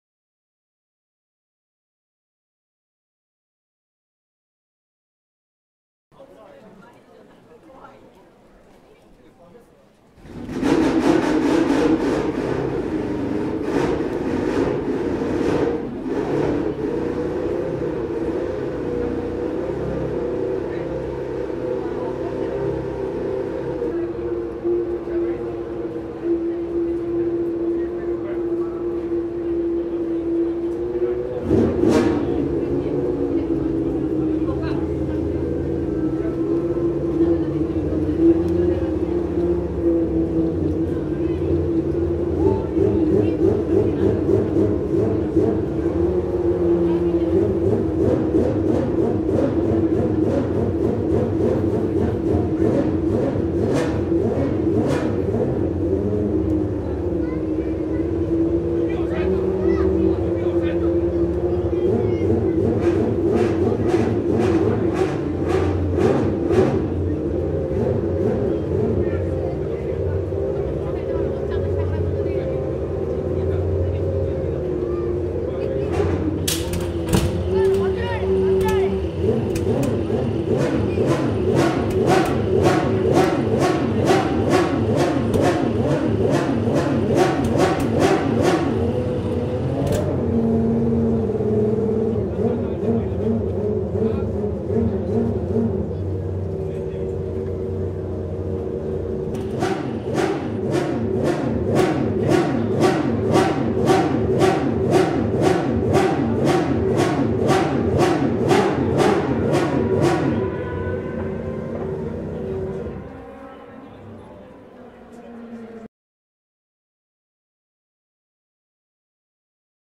ランボルギーニ F-1 V12 暖機 Lamborghini F-1 Start up
2012/3/3 鈴鹿サーキット開設50周年ファン感謝デー
エスポ・ラルース・ランボルギーニ　LC90(1990)
ミナルディ・ランボルギーニM192(1992)
Suzuka Circuit 50th Anniversary Mar.3.2012